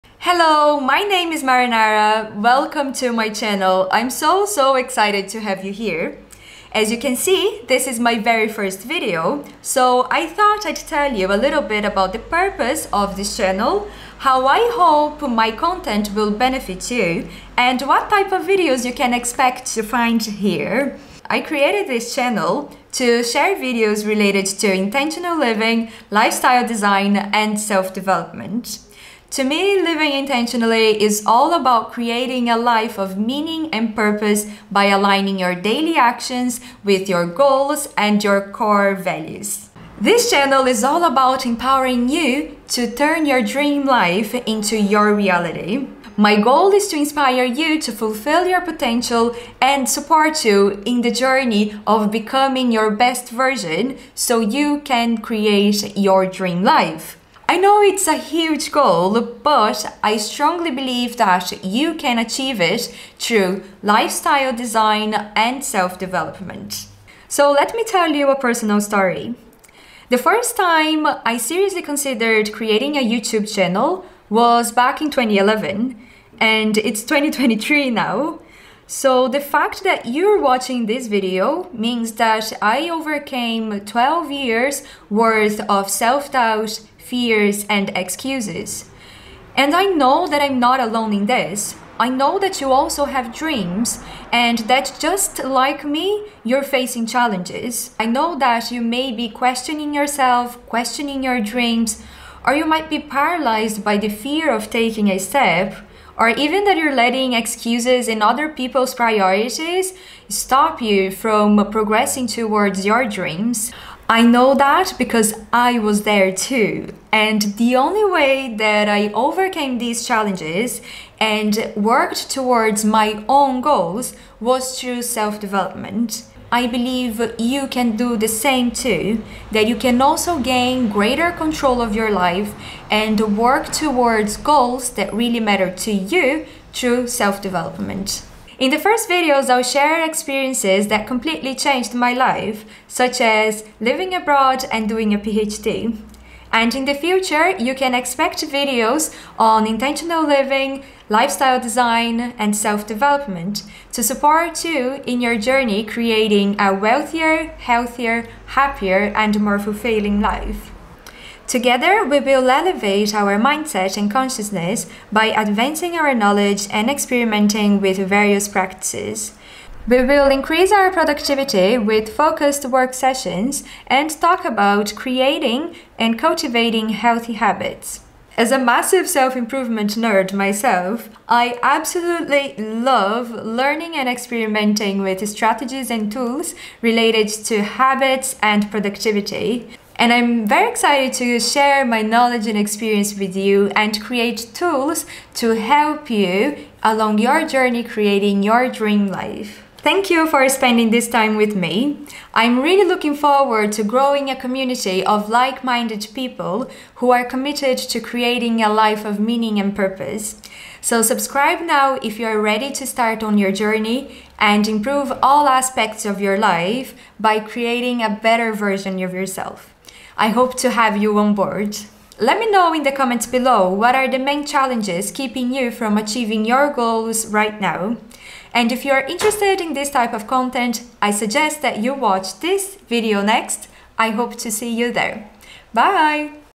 Turning dreams into reality | My channel’s purpose
This channel is all about turning dreams into your reality. My main goal is to inspire you to fulfil your potential and support you in the journey of becoming your best version so you can live the life of your dream life. I know this huge goal can be achieved through lifestyle design and self-development.

It took me 12 years to post this video - yes, I lived 12 years hidden behind self-doubt, fears and excuses. I took control over my life by designing a lifestyle according to my values and evolving to become the person I truly wanted to be. This did not happen overnight! Living intentionally is an ever-evolving process that allows us to find peace and fulfilment every day.

I hope my content helps you create the life of your dreams while enjoying your journey.

With love,
Mari!

Sejam bem-vindos a essa comunidade,

O propósito desse canal é transformar sonhos em sua realidade. Meu principal objetivo é inspirá-lo a realizar seu potencial e apoiá-lo na jornada de se tornar sua melhor versão para que você possa viver a vida dos seus sonhos. Eu sei que esse objetivo é um grande objetivo que pode ser alcançado com design de estilo de vida e do autodesenvolvimento.

Levei 12 anos para postar este vídeo - sim, vivi 12 anos me escondendo atrás de dúvidas, medos e desculpas. Assumi o controle da minha vida desenhando um estilo de vida de acordo com meus valores e evoluindo para me tornar a pessoa que realmente queria ser. Isso não aconteceu da noite para o dia! Viver intencionalmente é um processo em constante evolução que nos permite encontrar paz e realização todos os dias.

Espero que meu conteúdo ajude você a criar a vida dos seus sonhos enquando disfrutando da jornada.

Com amor,
Mari!

00:00 - Intro
00:21 - Why? Channel's purpose
01:07 - How? Channel's impact
02:18 - What? Channel's content
03:09 - Outro